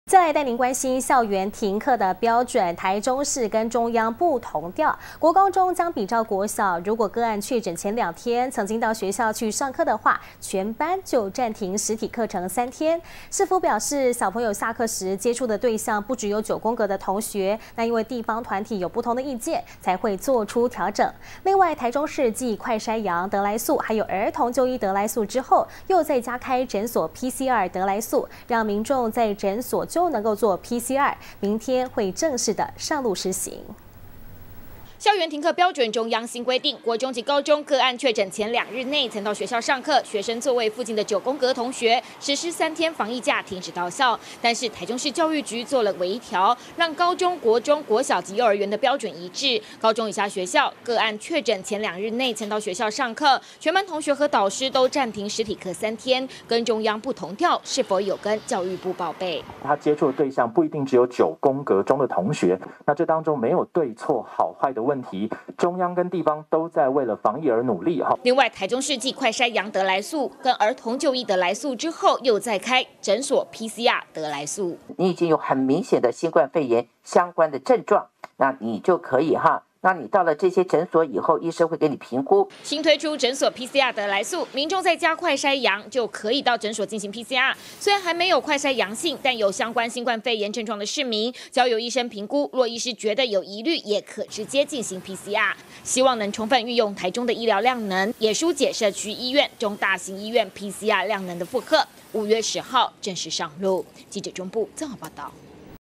不甩中央! 停課標準台中市與中央不同調
校園停課標準，台中市與中央不同調，國高中將比照國小，如果個案確診前2天曾經到學校上課，全班就暫停實體課程3天。市府表示孩子下課時接觸的對象不只有九宮格的同學，因為地方團體有不同意見，才會做出調整。另外，台中市繼快篩陽得來速跟兒童就醫得來速之後，又再開診所PCR得來速，讓民眾在診所就能作PCR，明天（5/10）正式上路施行。